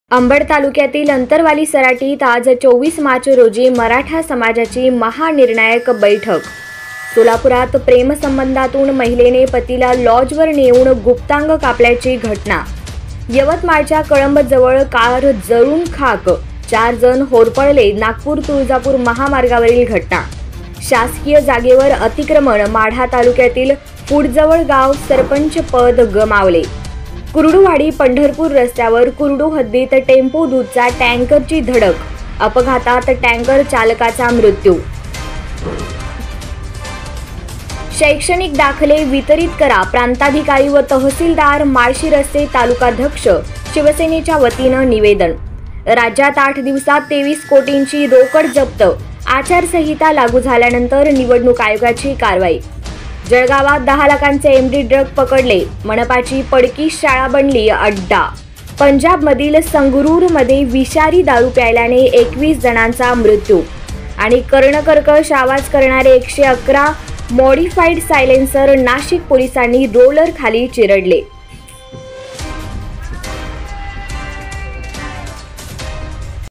PPR News | Top Ten Headlines | टॉप टेन हेडलाईन्स दि.24/03/2024
अंबड तालुक्यातील अंतरवाली सराटीत आज २४ मार्च रोजी मराठा समाजाची महानिर्णयाक बैठक

 सोलापुरात प्रेमसंबंधातून महिलेने पतीला लॉजवर नेऊन गुप्तांग कापल्याची घटना

 यवतमाळच्या कळंब जवळ कार जळून खाक, चार जण होरफळे नागपूर तुळजापूर महामार्गावरील घटना

 शासकीय जागेवर अतिक्रमण माढा तालुक्यातील फुटजवळगाव सरपंचपद गमावले

 कुर्डूवाडी-पंढरपूर रस्त्यावर कुर्डू हद्दीत टेम्पो दूधचा टँकरची धडक अपघातात टँकर चालकाचा मृत्यू

 शैक्षणिक दाखले वितरित करा प्रांताधिकारी व तहसीलदार माळशिरसचे तालुकाध्यक्ष शिवसेनेच्या वतीने निवेदन

राज्यात आठ दिवसांत 23 कोटींची रोकड जप्त; आचारसंहिता लागू झाल्यानंतर निवडणूक आयोगाची कारवाई

 जळगावात 10 लाखांचे एमडी ड्रग पकडले; मनपाची पडकी शाळा बनली अड्डा

 पंजाबमधील संगरुरमध्ये विषारी दारु प्यायल्याने 21 जणांचा मृत्यू

 कर्णकर्कश्श आवाज करणारे 111 मॉडिफाईड सायलेंसर नाशिक पोलिसांनी रोलरखाली चिरडले